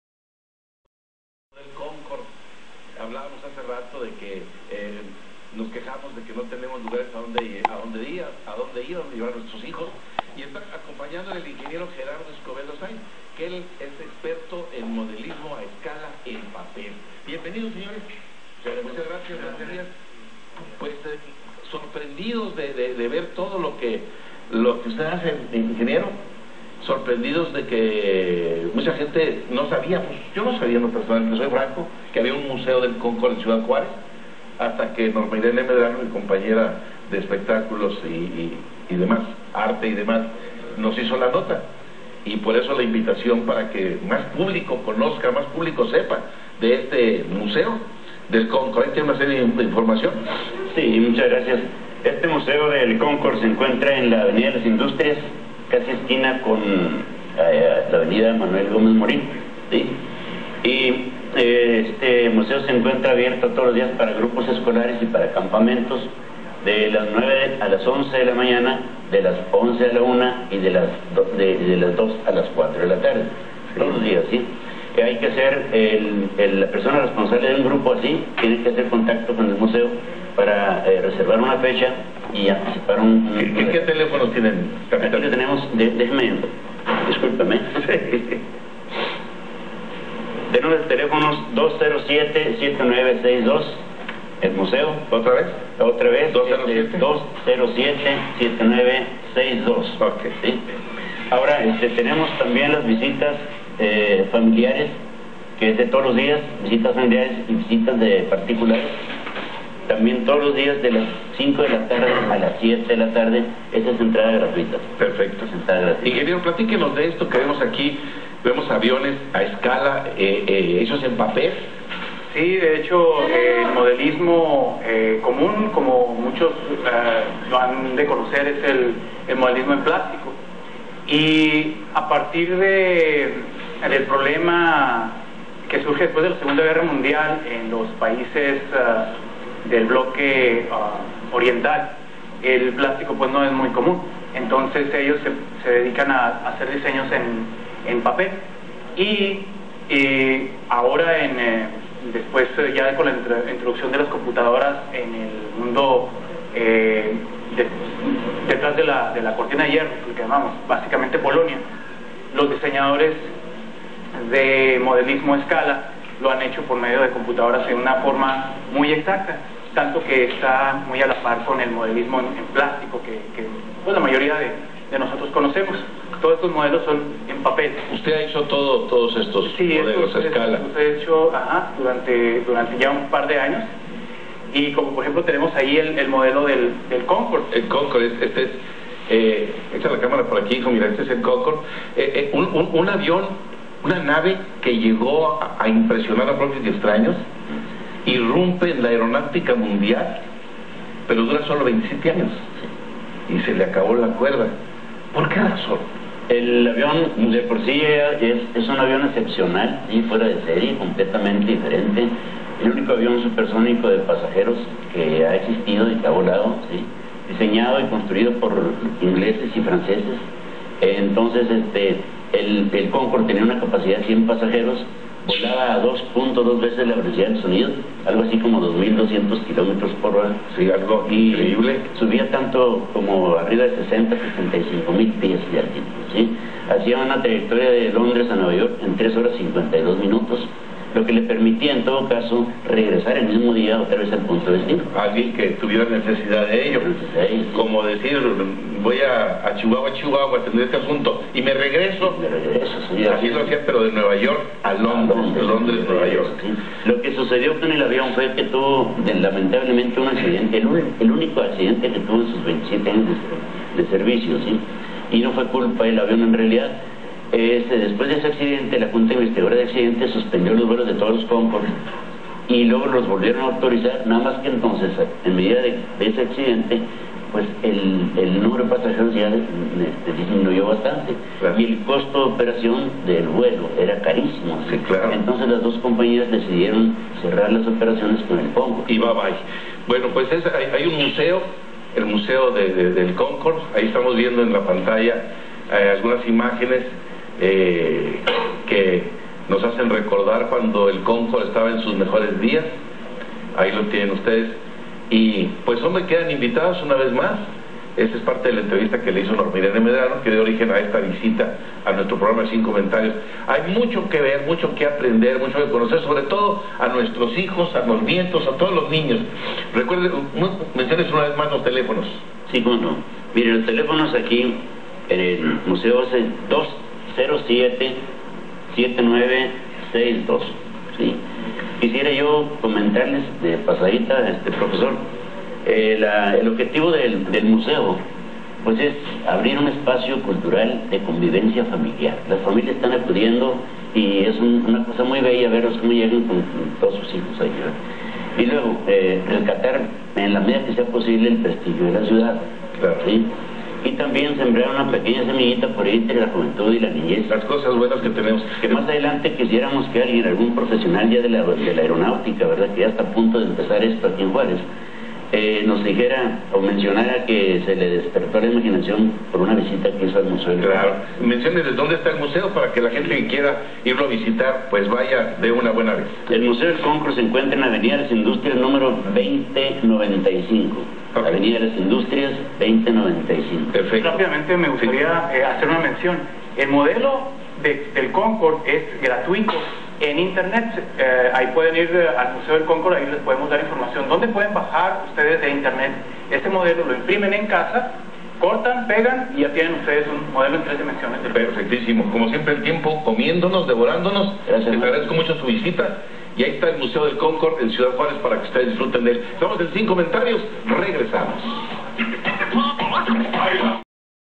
Entrevista Modelismo en Papel y Museo del Concorde
Entrevista en el canal 5 de Cd. Juárez, México; sobre modelismo a escala en papel y el Museo del Concorde.